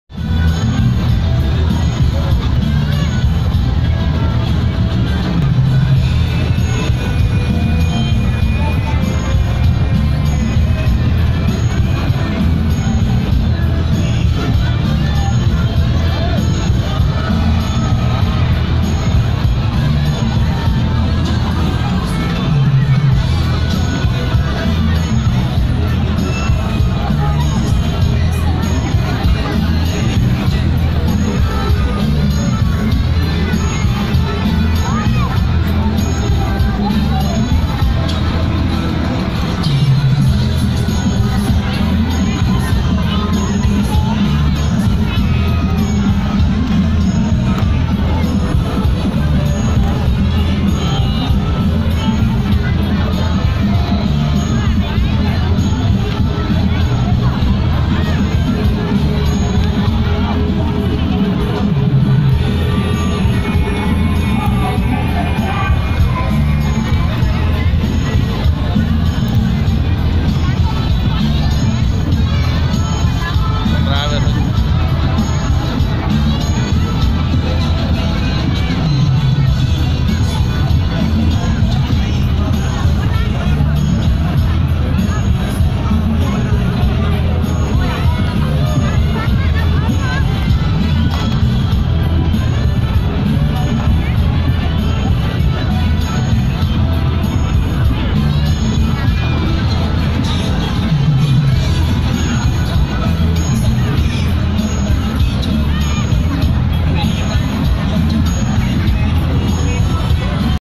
Children Car Video - Happy Children Play Small Car at Night Market Khok Pick Phnom Penh
Children Car Video - Children play car